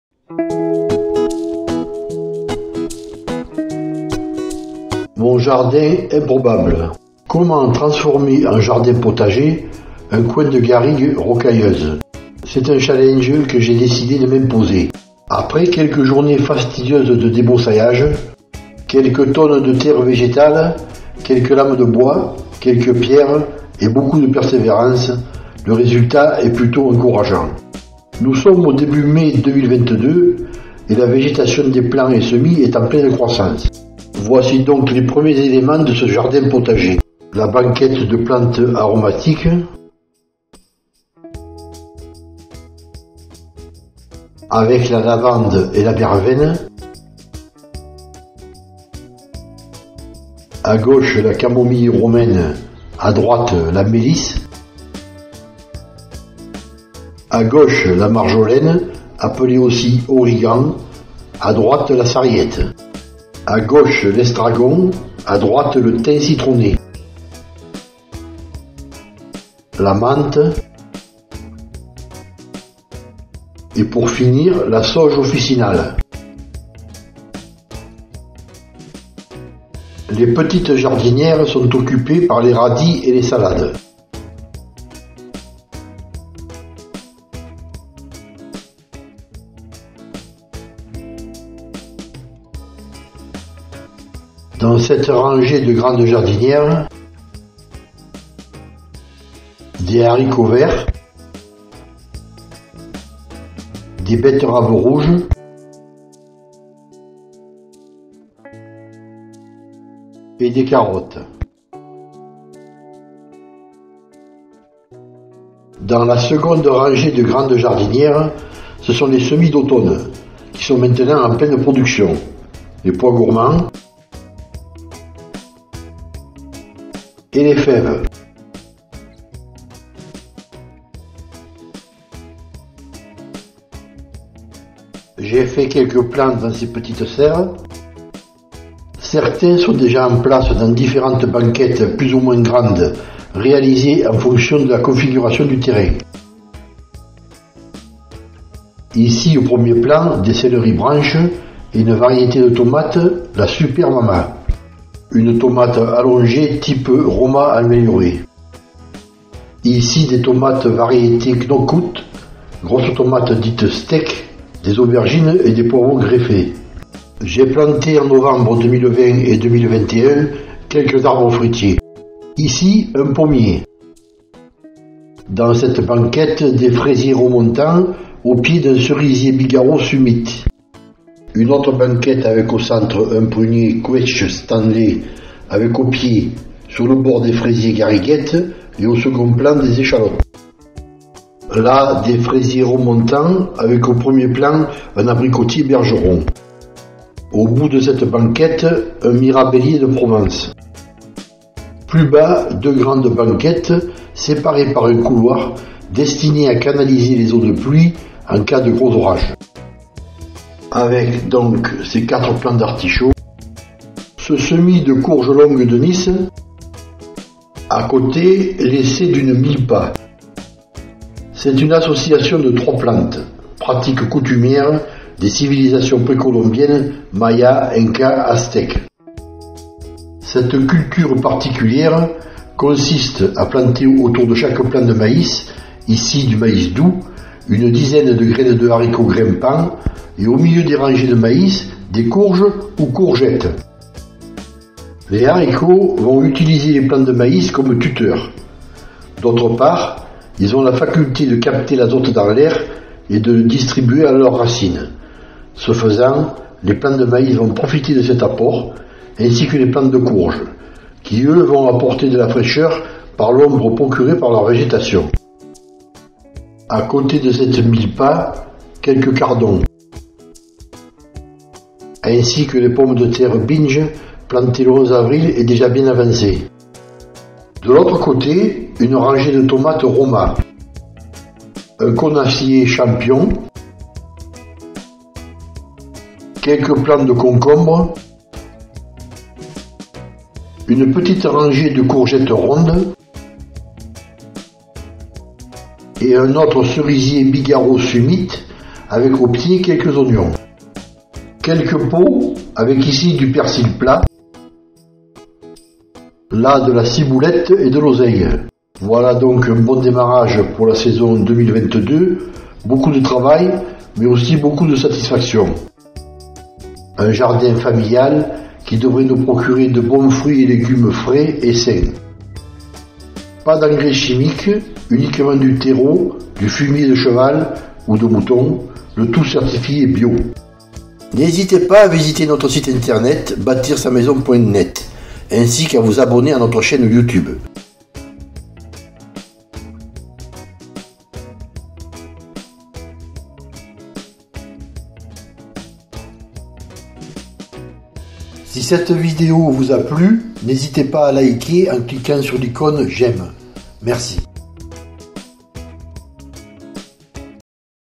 Mon Jardin Improbable
Vidéo : Mon Jardin Improbable
Comment  transformer en jardin potager un coin de garrigue rocailleuse ?
C’est un challenge que j’ai décidé de m’imposer !
Après quelques journées fastidieuses de débroussaillage.
Quelques tonnes de terre végétale, quelques lames de bois, quelques pierres, et beaucoup de persévérance, le résultat est plutôt encourageant !
Pour consulter la version écrite et illustrée rendez vous sur le blog :
« j’aime »  Merci !